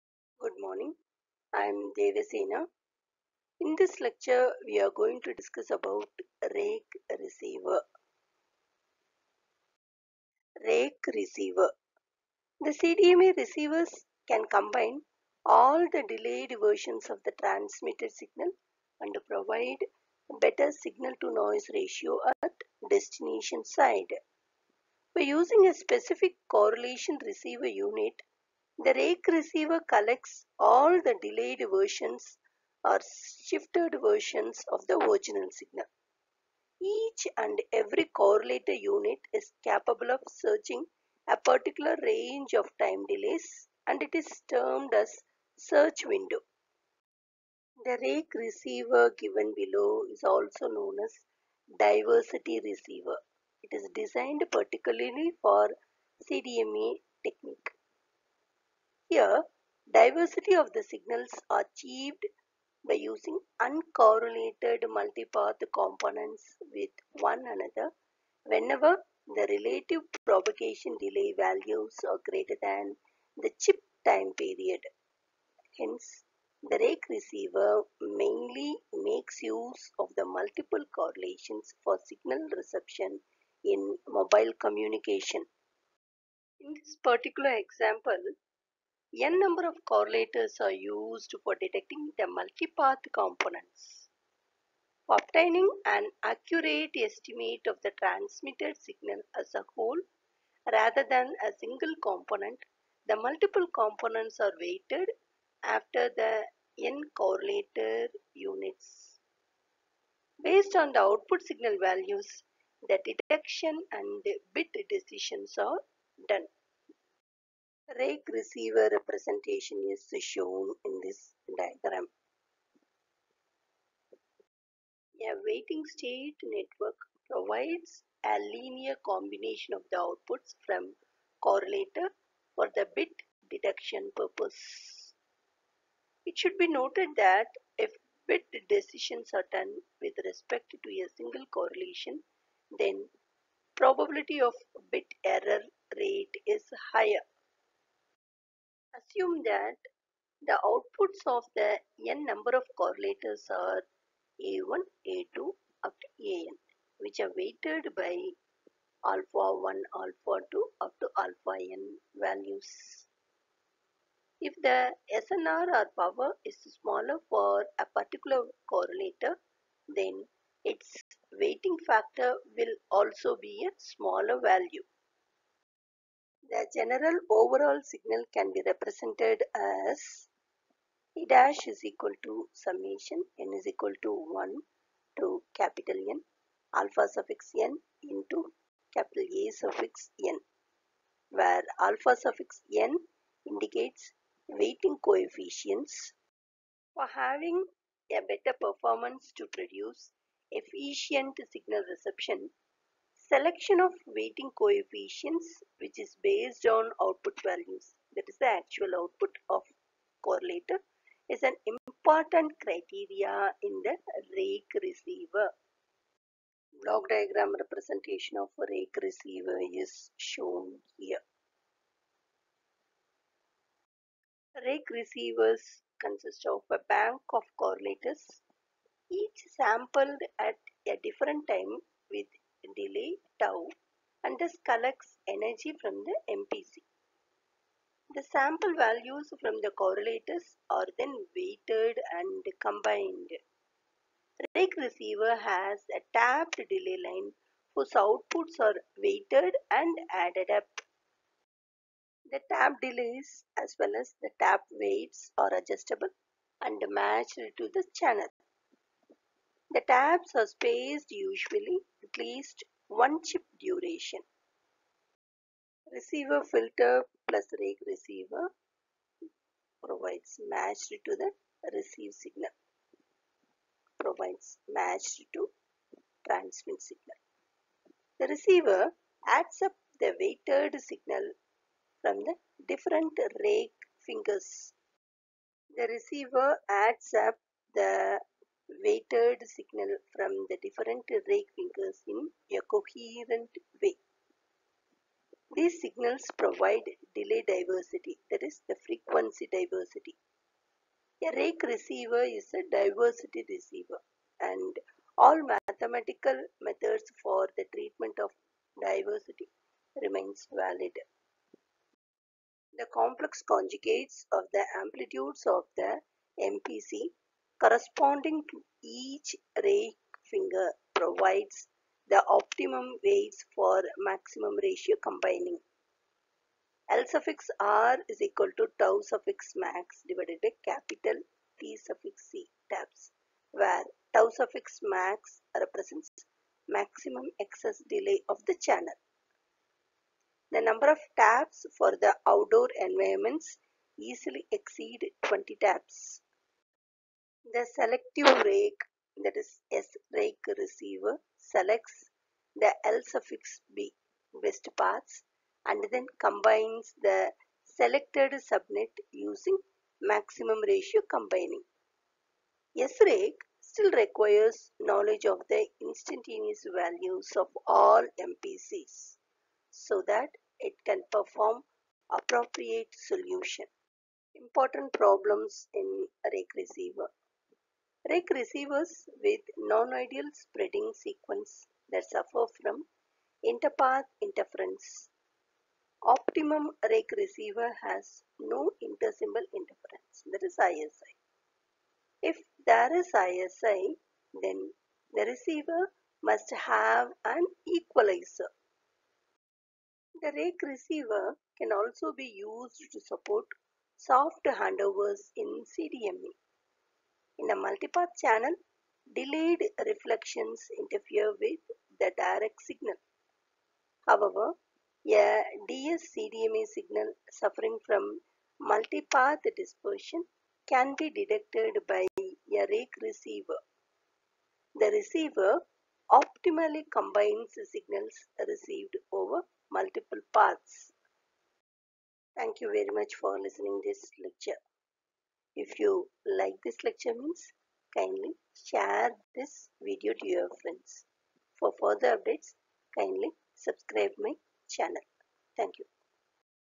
RAKE Receiver
The CDMA receivers can combine all the delayed versions of the transmitted signal and provide better signal to noise ratio at destination side.
By using a specific correlation receiver unit, the “RAKE” receiver collects all the delayed versions/shifted versions of the original signal.
Each and every correlator unit is capable of searching a particular range of time delays and it is termed as “search window”.
The RAKE receiver given below also known as “Diversity receiver” designed particularly for CDMA technique.
Here diversity of the signals are achieved by using uncorrelated multipath components with one another whenever the relative propagation delay values are greater than the chip time period.
Hence the “RAKE Receiver” mainly makes use of the multiple correlations for signal reception in mobile communication.
In this  particular example “N” number of correlators are used for detecting the multipath components.
For obtaining an accurate estimate of the transmitted signal as a whole, rather than a single component, the multiple components are weighted after the ‘n’ correlators units.
Based on the output signal values the detection and the bit decisions are done.
A weighting state (network) provides a linear combination of the outputs from correlator for the bit detection purpose.
It should be noted that if bit decisions are done with respect to a single correlation then probability of bit error rate is higher.